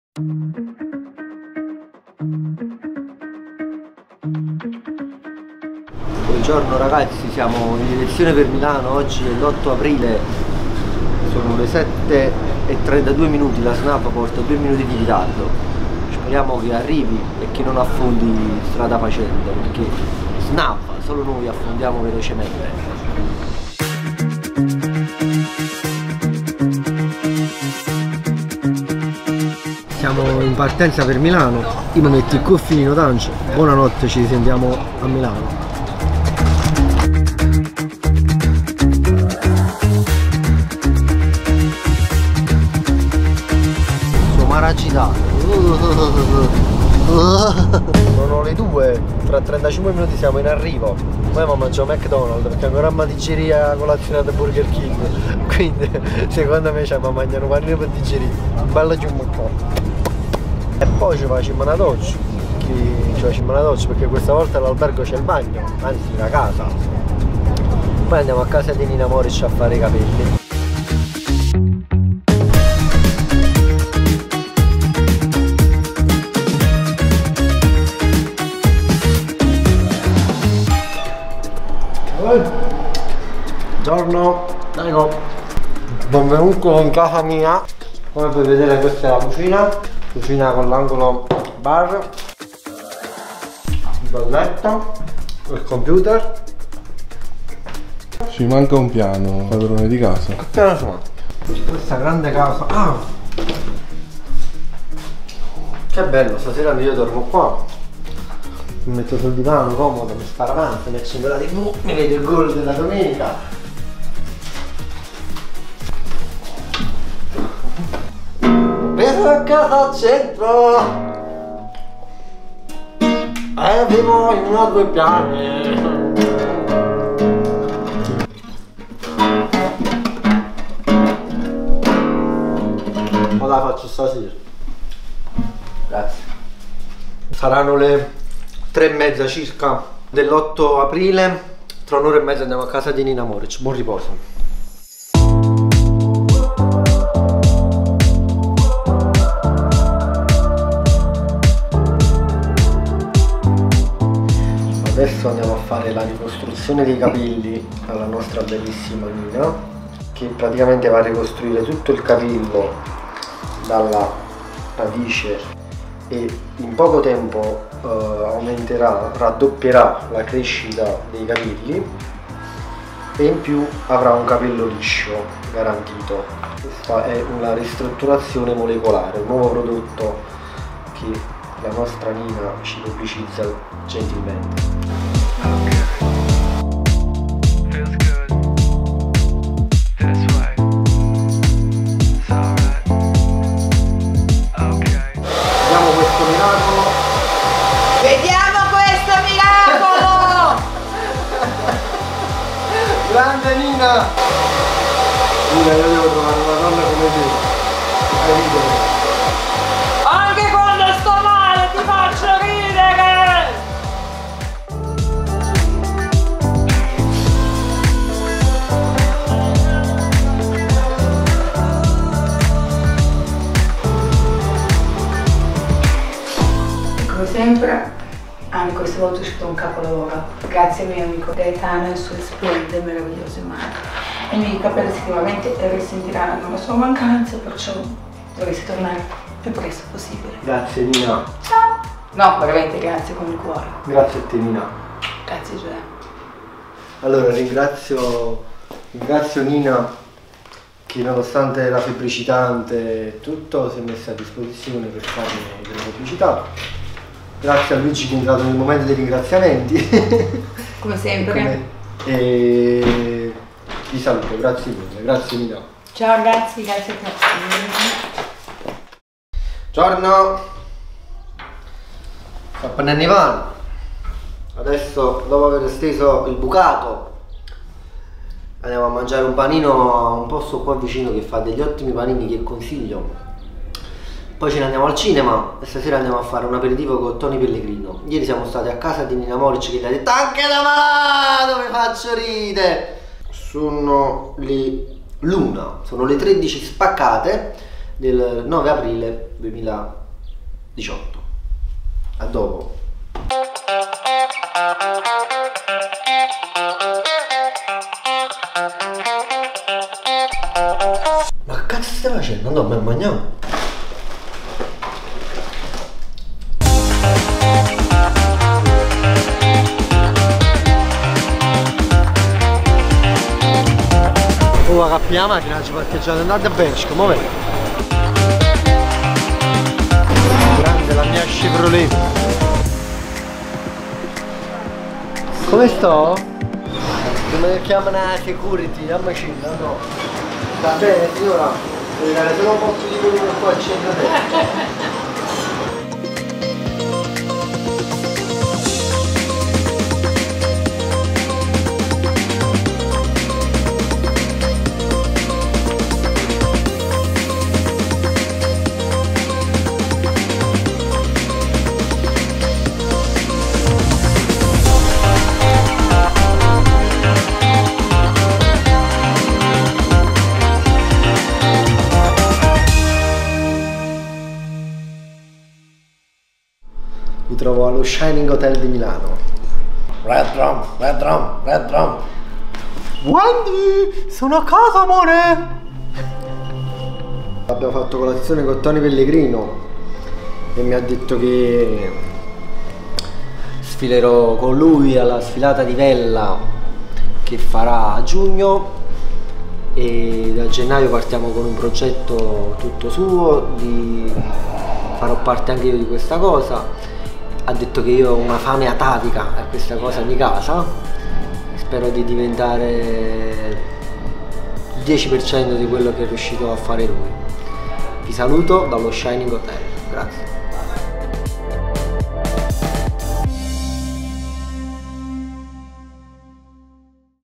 Vlog#2 Nina Moric Milano Aprile 2018
Dopo quasi 4 anni, condivido con voi le mie avventure in giro per l'Italia!
Questa è il mio secondo vlog a Milano, di nuovo a casa di Nina Moric.
Buon viaggio ;)